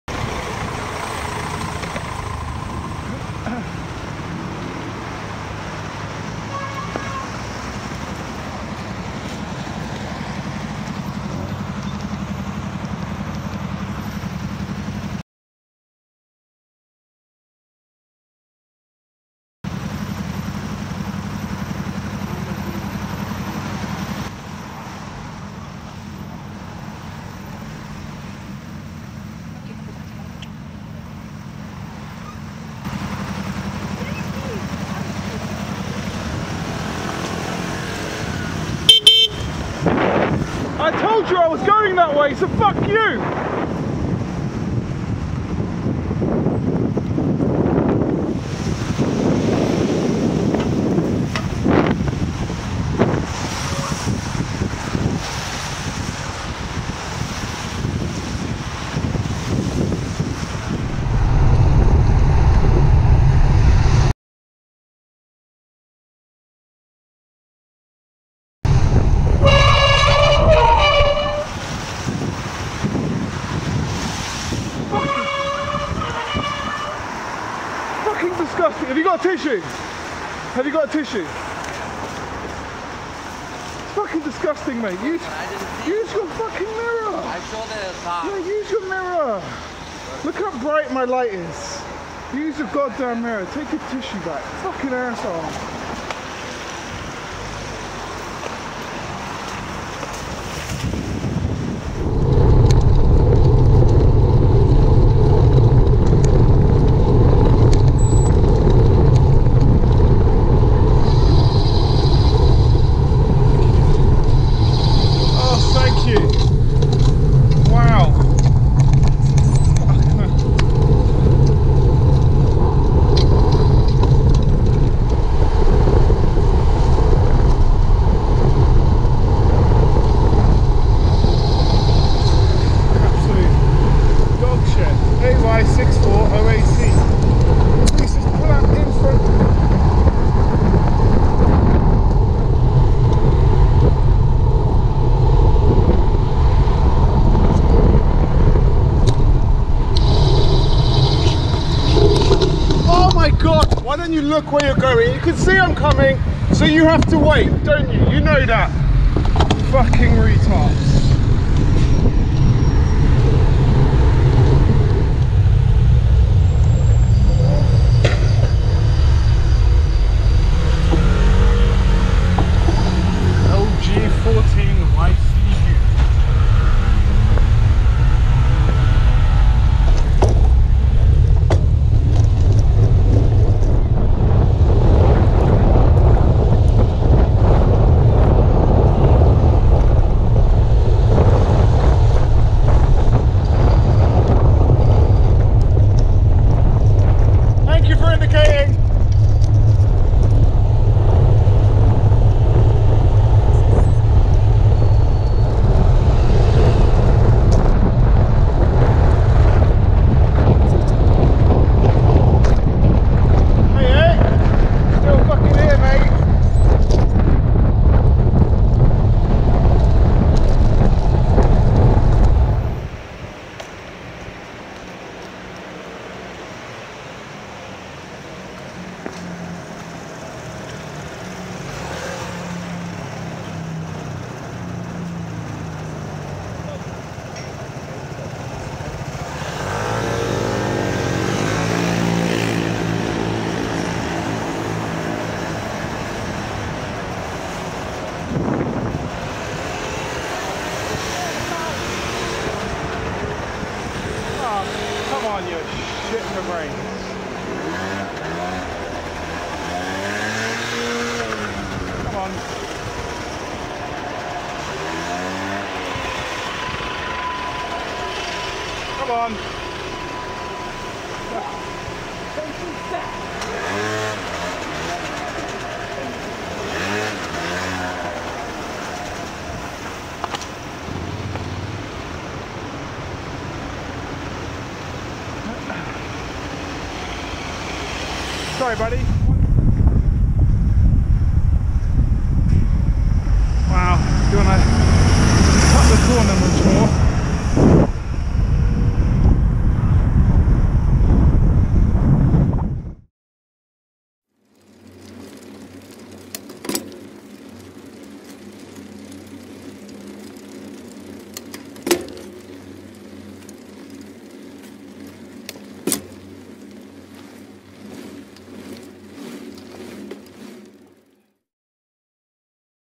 Mildly Annoying Driving
A few things that are mildly annoying or just down right dangerous. However none reported to the police for various reasons.